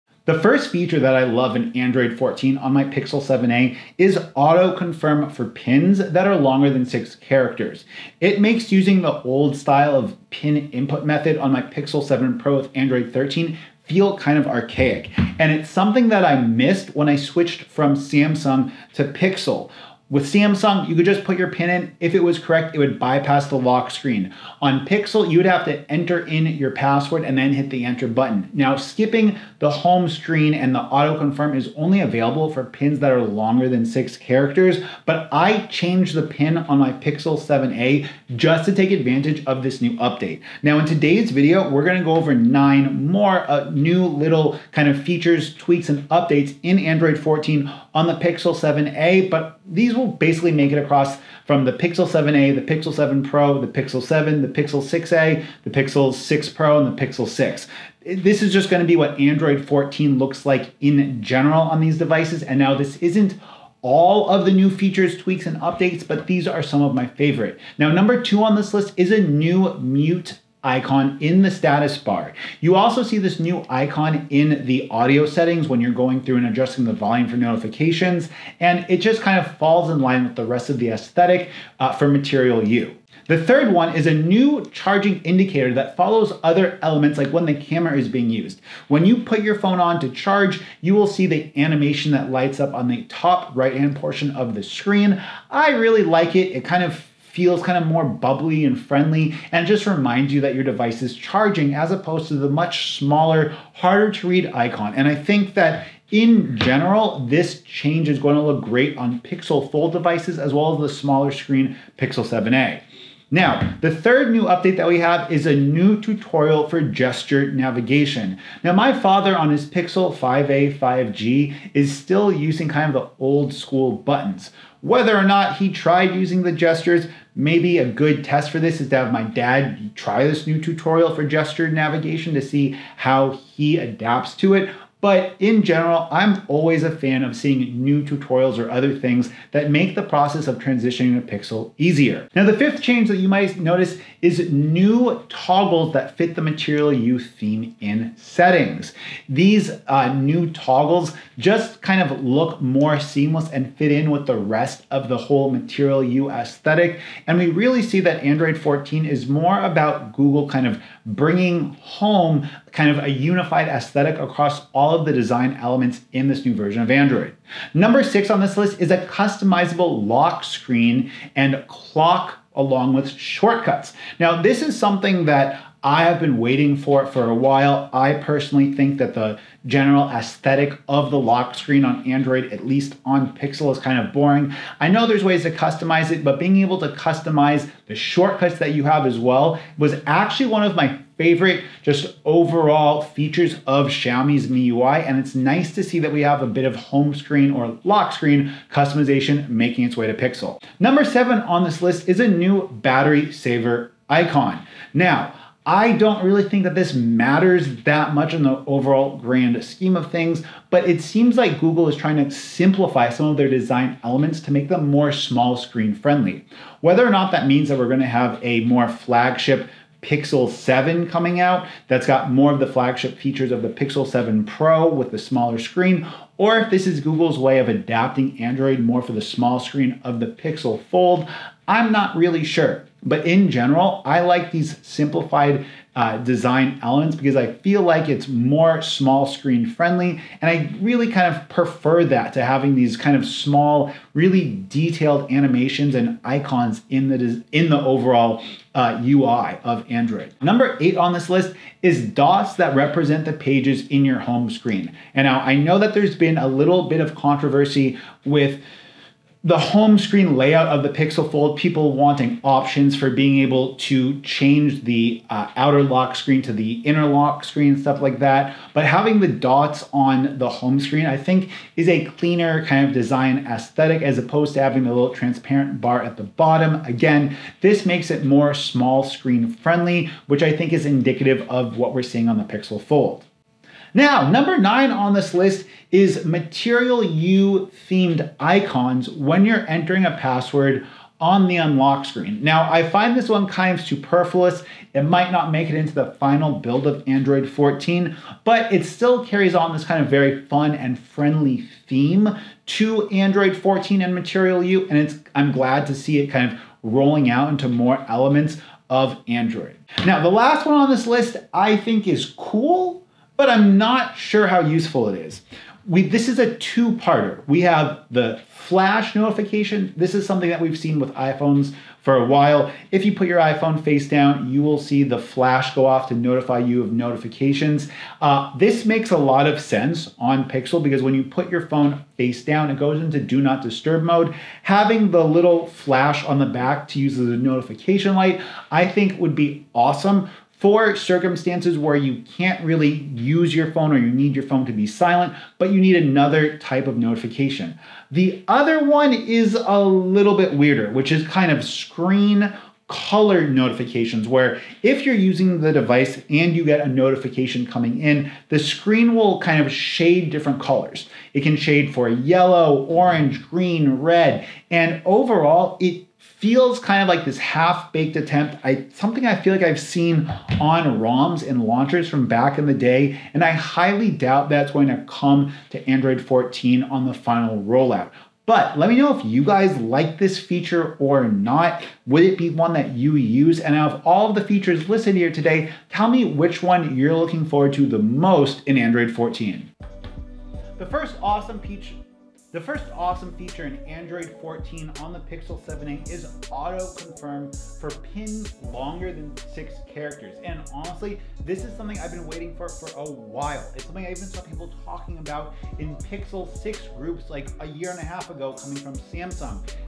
10 Features Of Android 14 On The Pixel 7A
Android 14 is right around the corner and with the launch of the pixel fold and recent launch of the pixel 7a lots of people are wondering what the future of android is going to look like. Luckily i have been using android 14 on my pixel 7a for a few weeks now and the recent improvements with android 14 seem to be pretty minor. While android 14 might have some under the hood improvments overall we see that android 14 seems to be aimed at smaller screens. This makes sense since t he pixel fold uses a smaller outer display and the pixel 7a has the smallest display of any pixel device to date. After using android 14 on my pixel 7a i feel that android 14 is a subtle enhancements that has a few